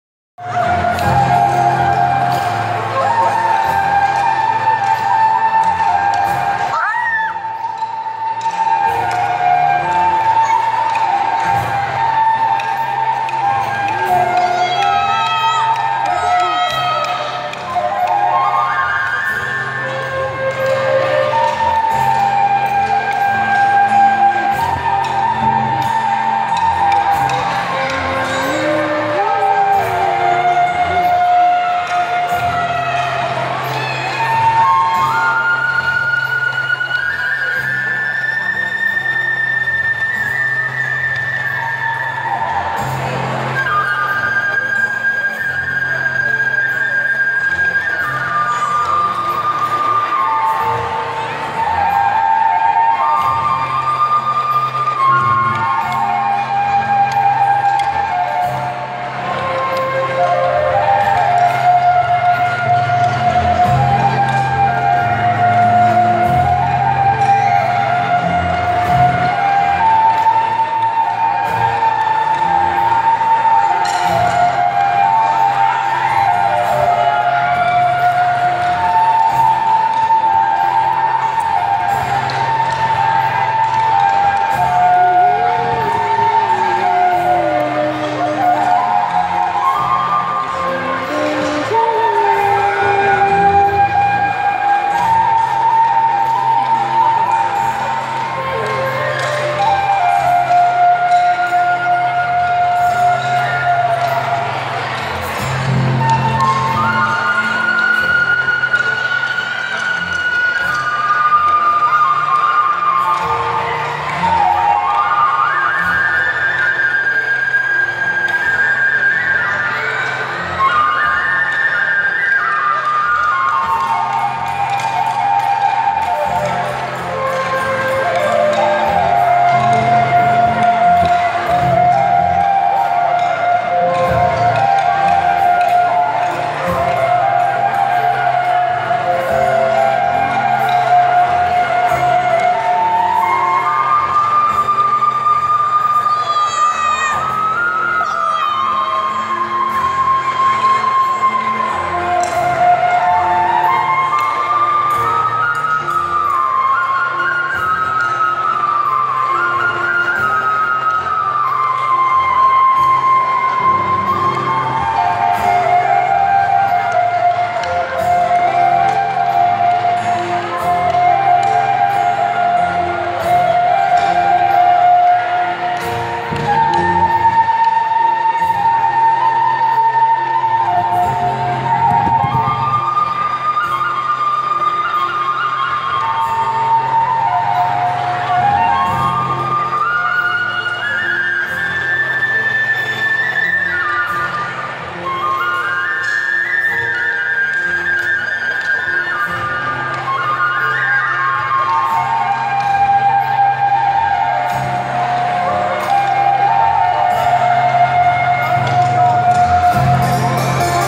Navratri 2017, Camarillo | Flute
The best Navratri, organized by Ventura Indian Community Association (VICA) with the whole fun, lively and brilliant musical team!